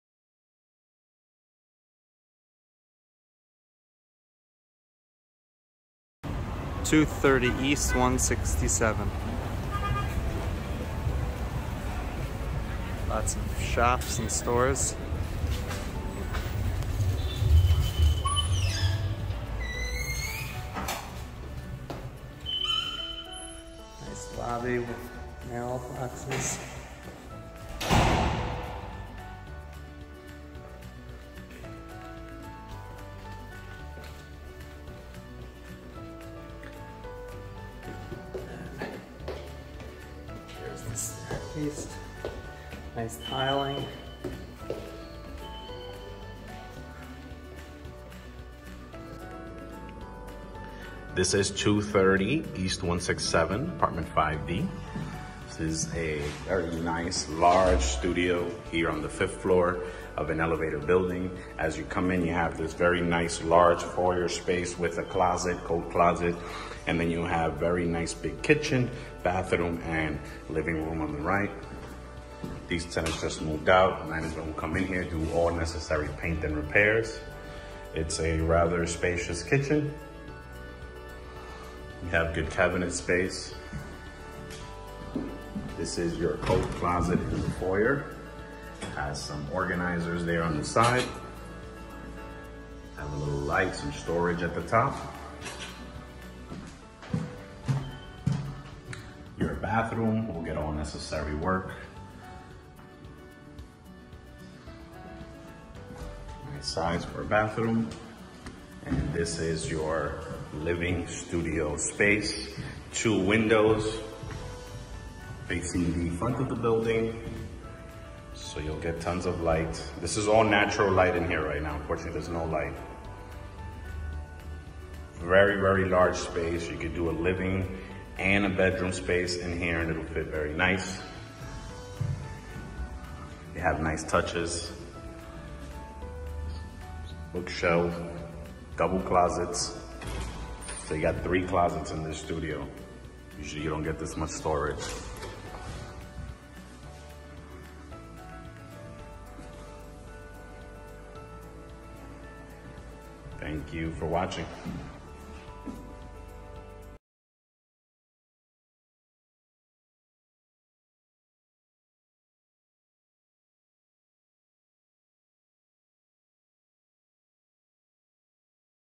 230 East 167th St. 5D (Studio)
Beautiful Studio apartment in Concourse, Bronx.